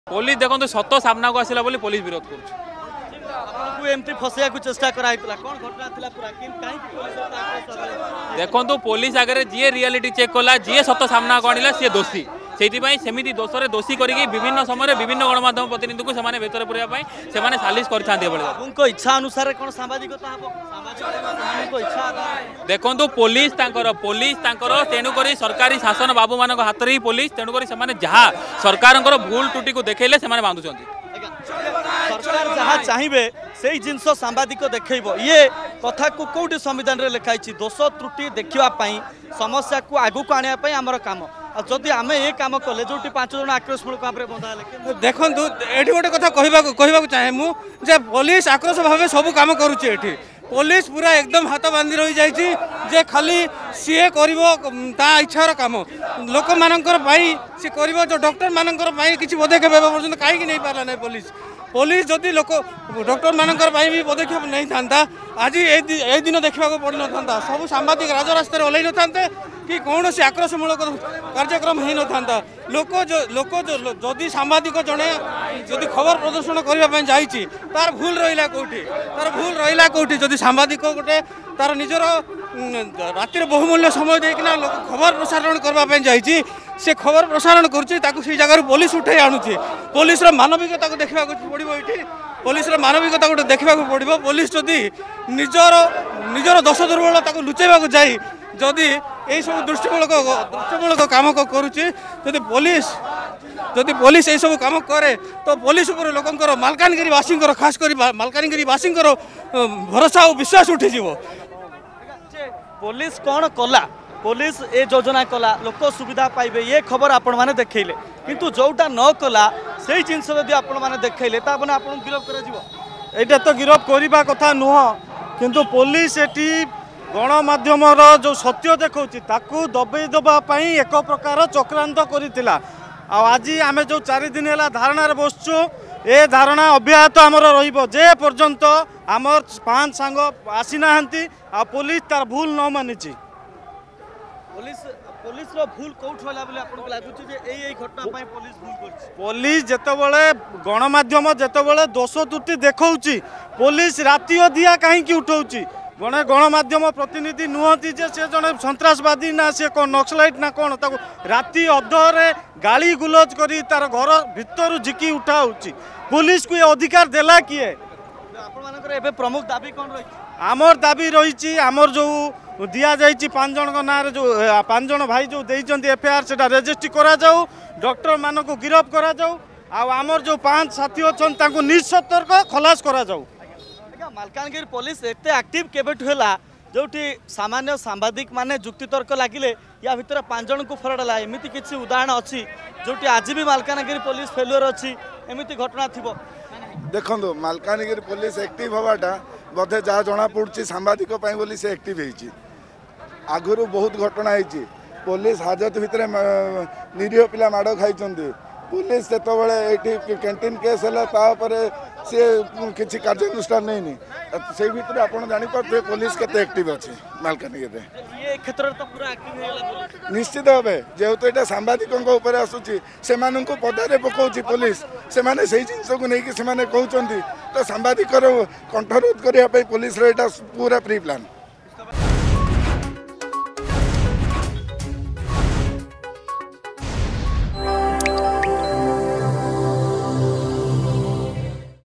ସତ ସାମ୍ନାକୁ ଆସିଲା ବୋଲି ପୋଲିସ ଗିରଫ କଲା : ସାମ୍ବାଦିକ ସଂଘ
ମାଲକାନଗିରି ଜିଲ୍ଲା ର ପାଞ୍ଚଜଣ ସାମ୍ବାଦିକଙ୍କ ଉପରେ ମିଛ ଅଭିଯୋଗରେ ପୋଲିସ ଗିରଫ କରିବା ପ୍ରତିବାଦ କରିଛି ମାଲକାନଗିରି ସାମ୍ବାଦିକ ସଂଘ ।